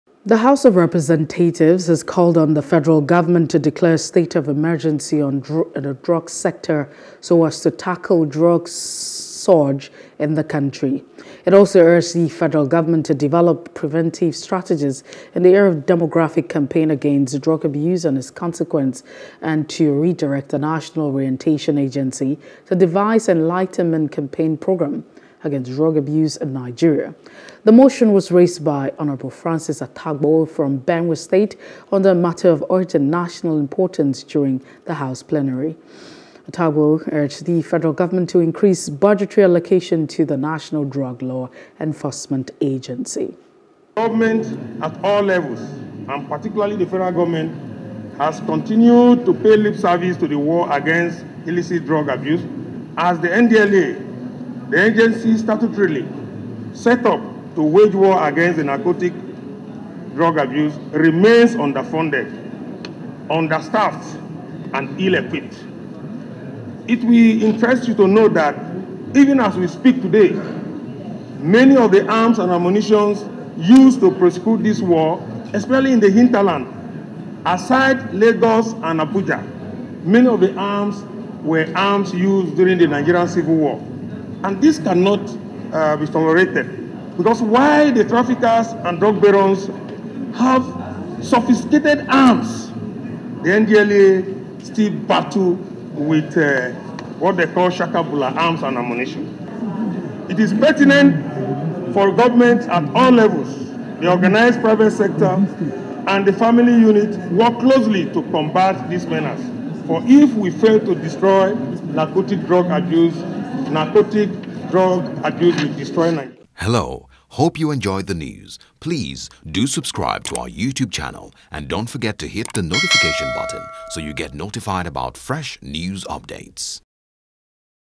Drug Abuse: Reps Urge F.G. To Declare State Of Emergency On Drug Abuse
The House of Representatives has called on the Federal Government to declare a state of emergency on the drug sector so as to tackle drug scourge in the country.